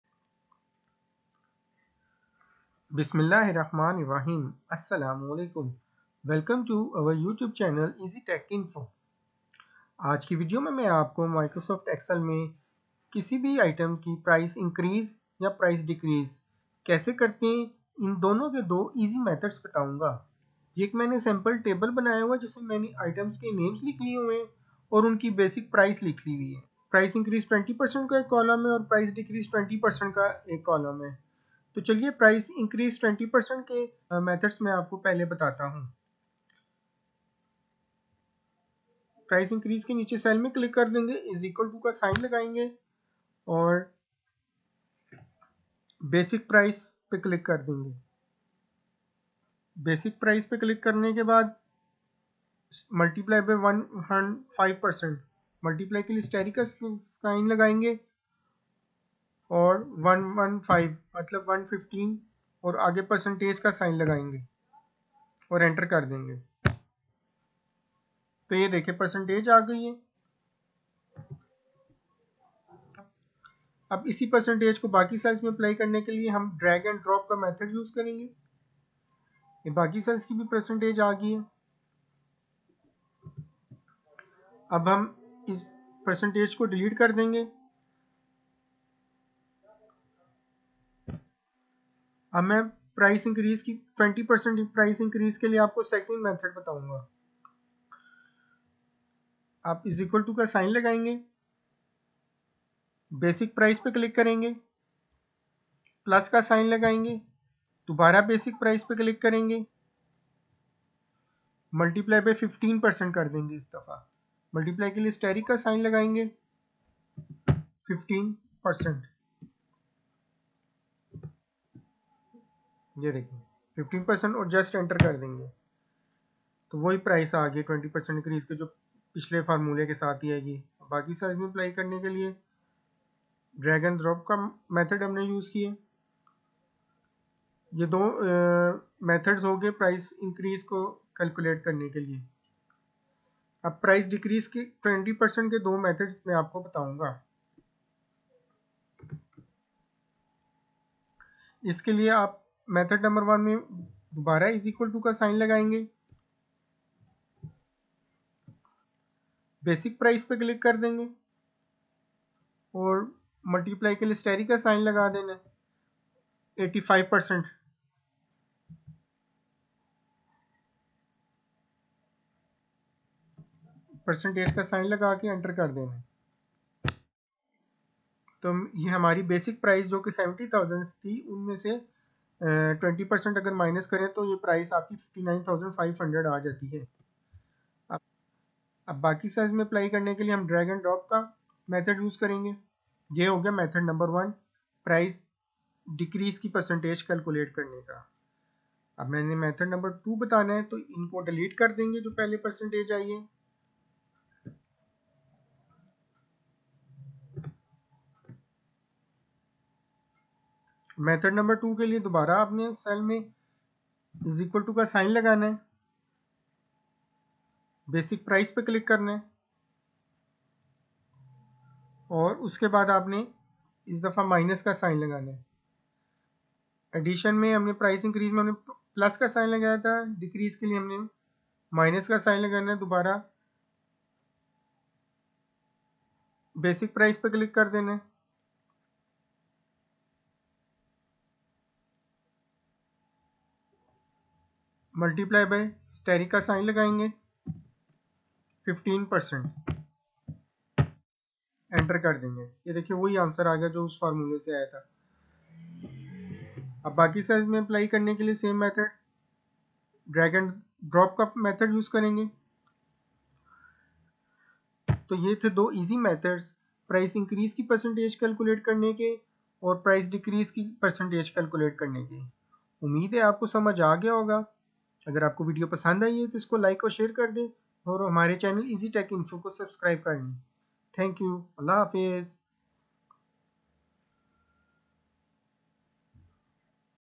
Easy Methods to Calculate Percentage Increase & Decrease in MS Excel | Easy Tech Info
Easytechinfo, Easy Tech Info, Easy techinfo

In this video we will learn two easy methods to calculate Percentage Increase & Decrease in MS Excel